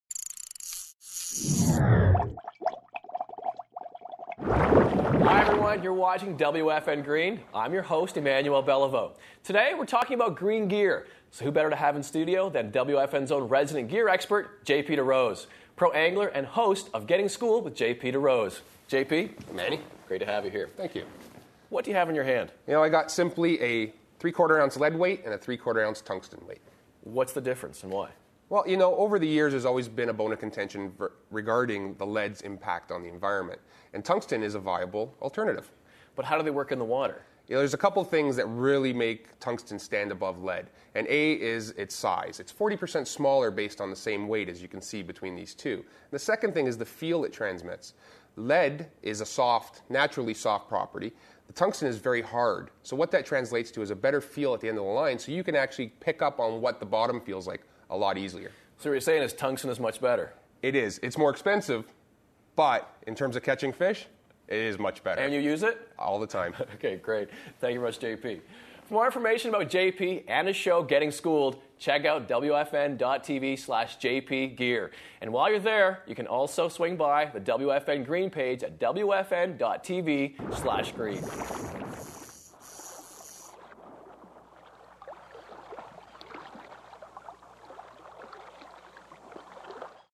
Lead vs Tungsten
JP DeRose talks about the differences between lead and tungsten.